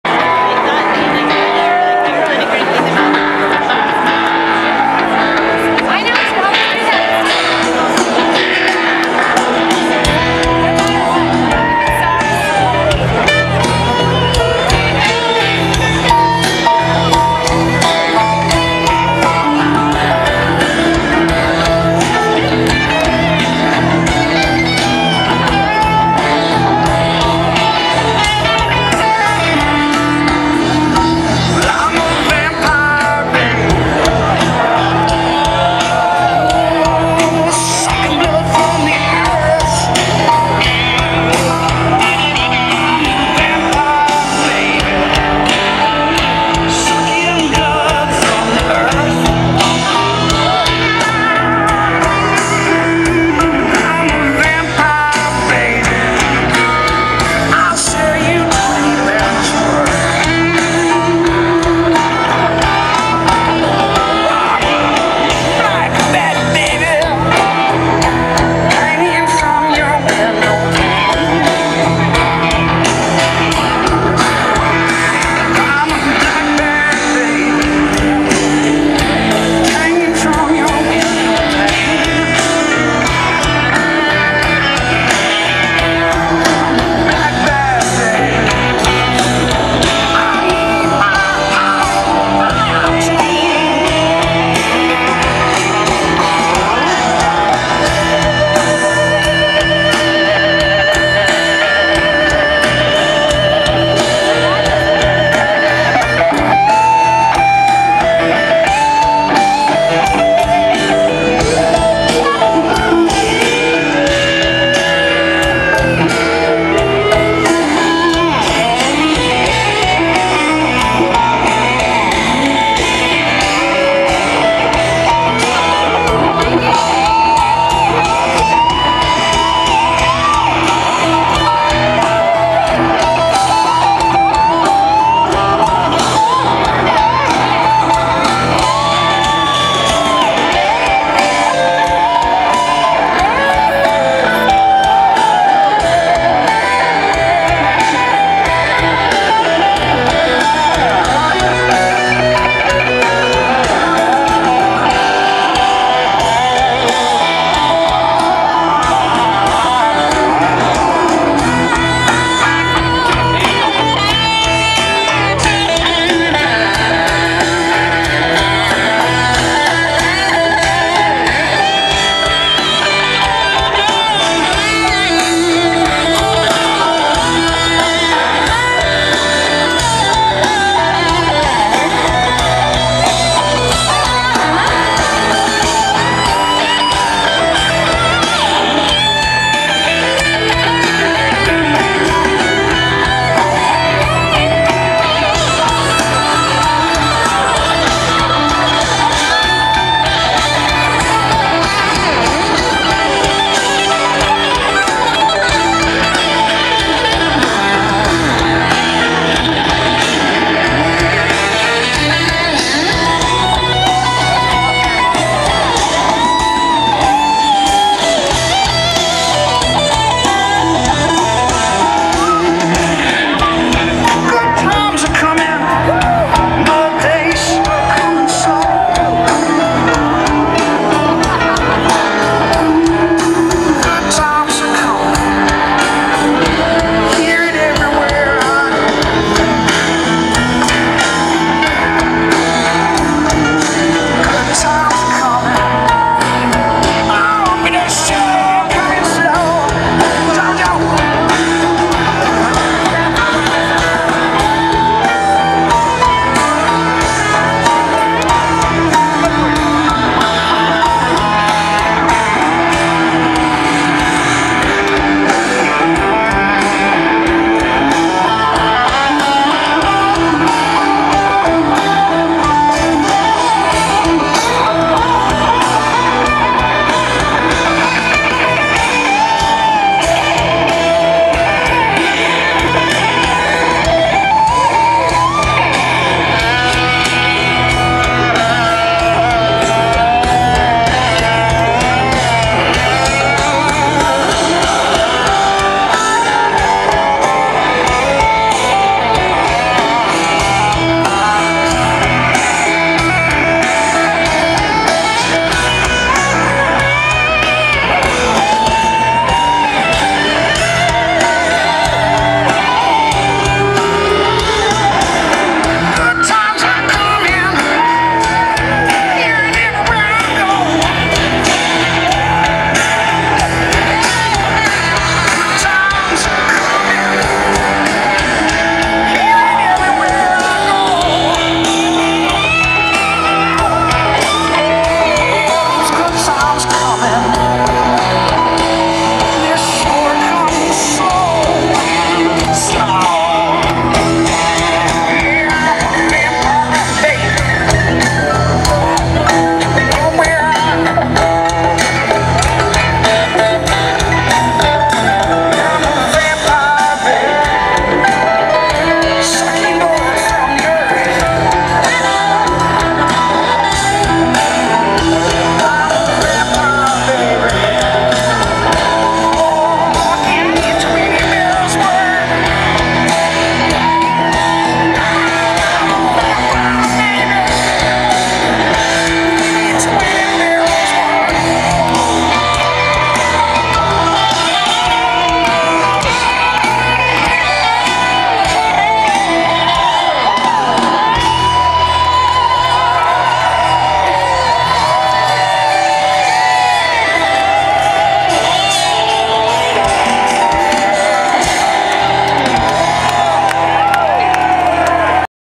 Widespread Panic at Red Rocks 2008
Widespread Panic performs at Red Rocks... 6/27/2008...